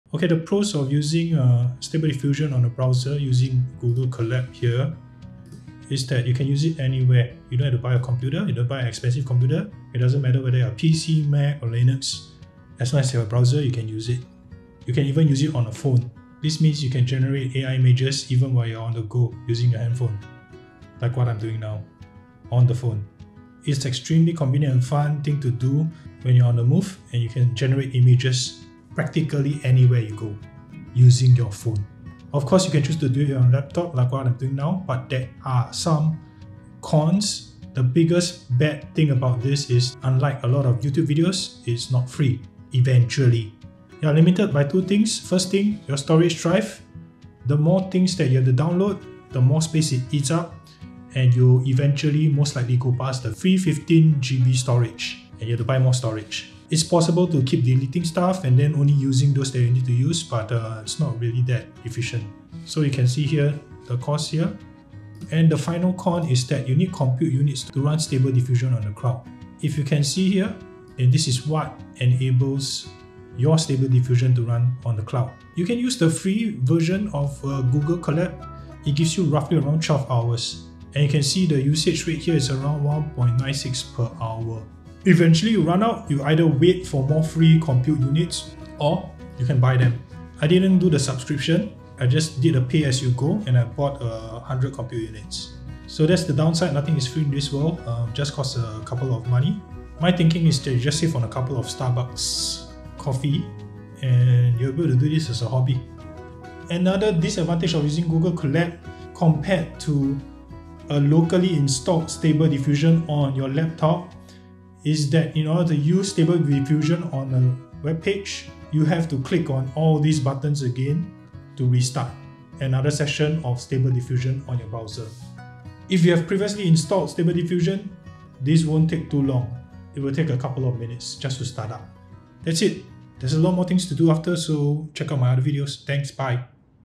Simple fast easy AI art Stable Diffusion 02: It's not really free. Google Collab SD Pros and Cons
Don't believe what other youtube videos say about stable diffusion being free if you use Google Collab. Yes, it's free at first, but eventually you'll have to fork out a little $$$ to keep it going. Small $ but it's okay for a new exciting hobby like this yeah? Plus other pros and cons of using stable diffusion in google collab!